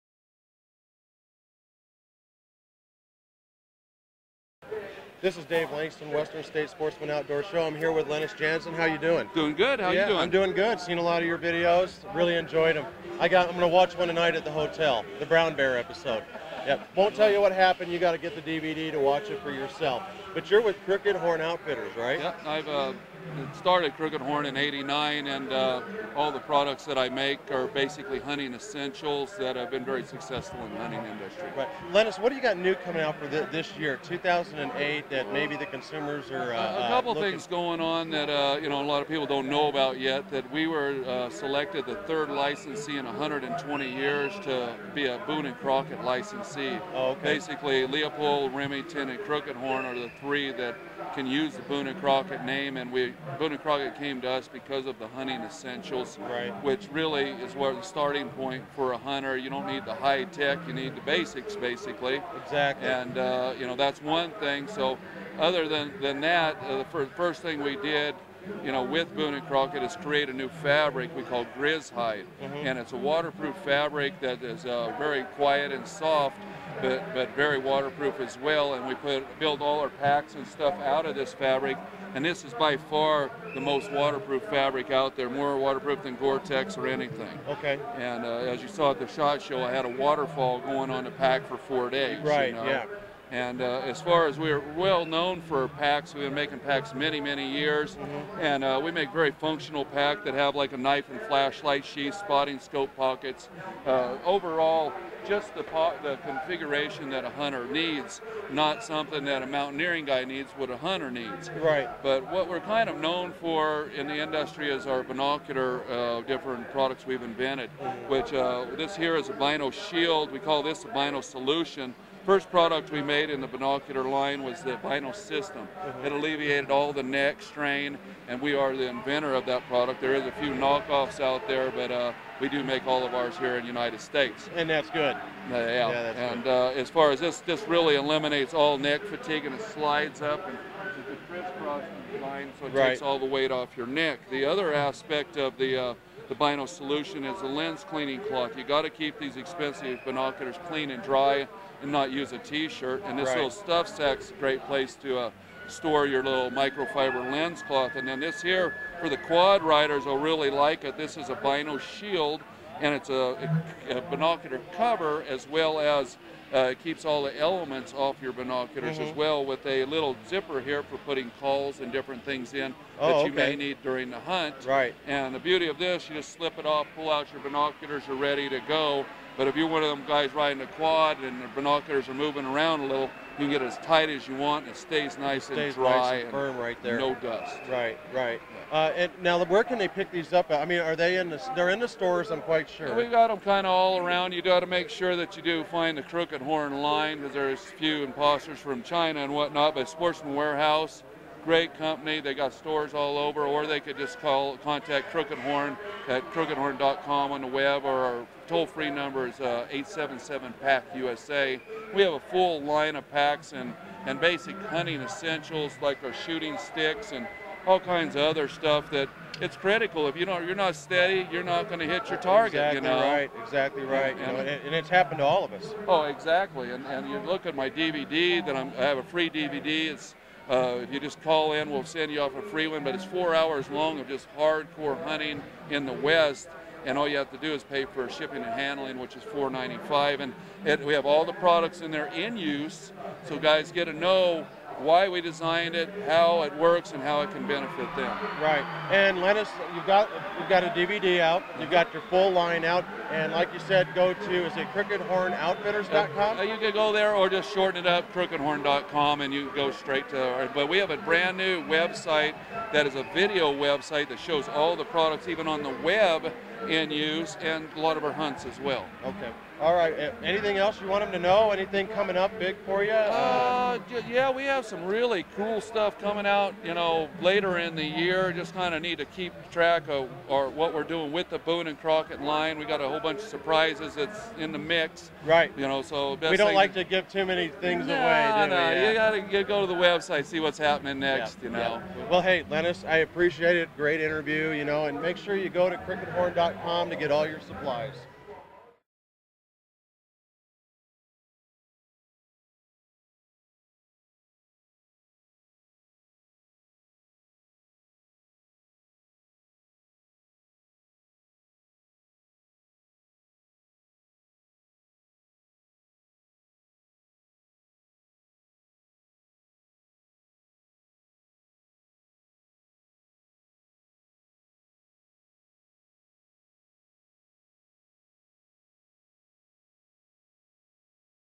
Crooked Horn Outfitters interview with Dave Langston
Interview with Crooked Horn Outfitters and owner and TV host Lennis Janzen.